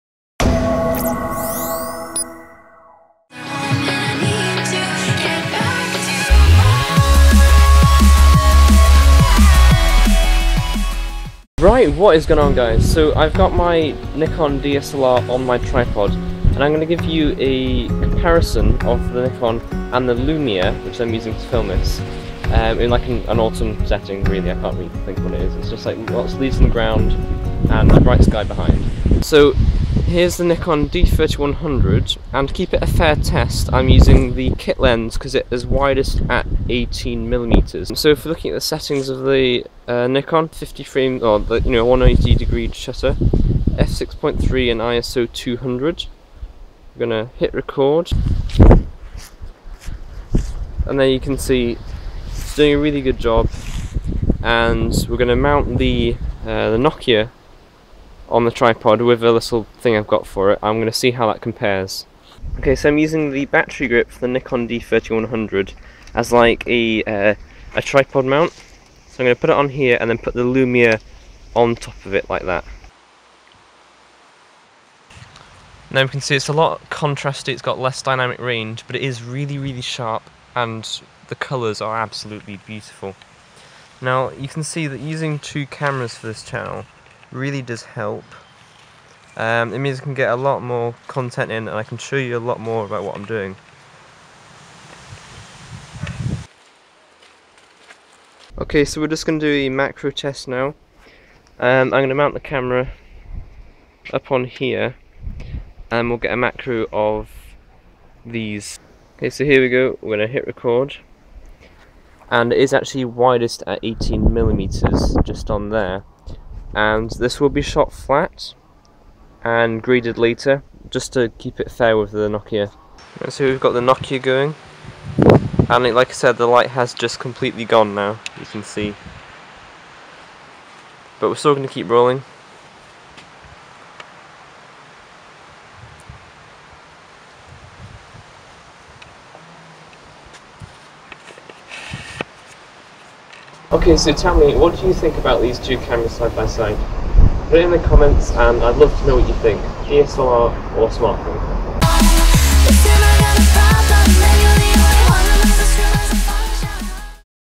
Nokia Lumia 920 Vs DSLR | Video Quality Comparison
Powered By Fullscreen.net™

In this video i compare the Nokia Lumia 920 and the Nikon D3100 DSLR in terms of video quality. The Lumia 920 is my new B camera and is a strong contender in video performance. The video from it is sharp, contrasty and full of colour. However it has a very low dynamic range compared to the D3100.

~Jason Host Of TheXtremesoundz Audio & Vision.